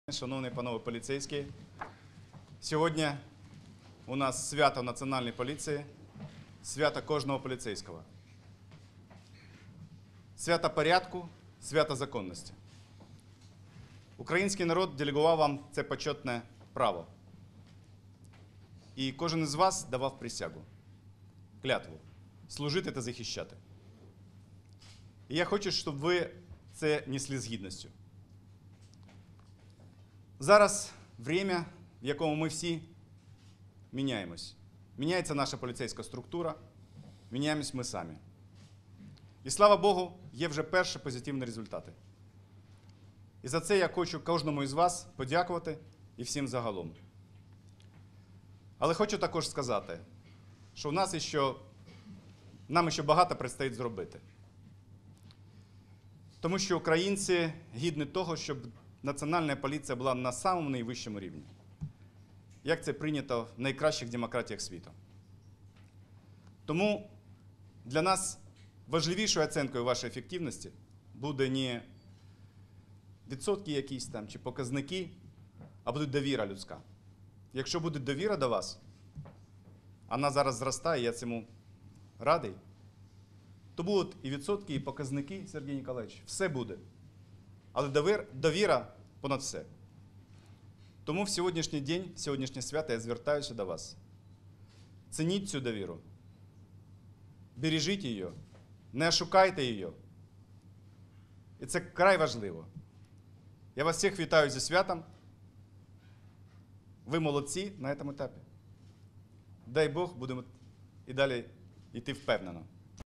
Аваков у День Нац.поліції України: Той, хто не бачить змін, не хоче їх бачити
Міністр внутрішніх справ, представник партії «Народний фронт» Арсен Аваков привітав поліцейських України з професійним святом і подякував їм за участь у становленні української поліції, яка завойовувала довіру громадян.

«Сьогодні свято Національної поліції, свято кожного поліцейського. Це свято порядку й законності. Кожен з вас давав клятву «Служити та захищати». І я хочу, щоб ви виконували свій обов’язок з гідністю. Зараз час змін: змінюється поліцейська структура, змінюємося ми самі. І вже є перші позитивні результати, за які я вдячний кожному з вас окремо і всій поліції загалом», - наголосив він.

«Але у нас багато роботи попереду. Адже українці варті тощо, щоб наша поліція була на найвищому рівні, як у найкращих демократіях світу. Пам’ятайте, що найважливішою оцінкою діяльності поліцейських є довіра українців. Тому в цей день я звертаюся до вас: цінуйте цю довіру, бережіть її!» – наголосив Арсен Аваков.

Цього дня 60 кращих працівників різних підрозділів поліції з різних куточків України були нагороджені Почесними грамотами Кабінету Міністрів, Подяками Прем’єр-міністра України, Грамотами Верховної Ради та відомчими заохочувальними відзнаками МВС.

Окремо Арсен Аваков підкреслив, що Нацполіція продовжує рухатися визначеним шляхом реформування. «Той, хто не бачить змін, той не хоче їх бачити. Ми не працюємо для них. Ми працюємо для Українського народу. І працюємо так, щоб не було соромно кожному з нас», – заявив очільник МВС.

Приєднуйся!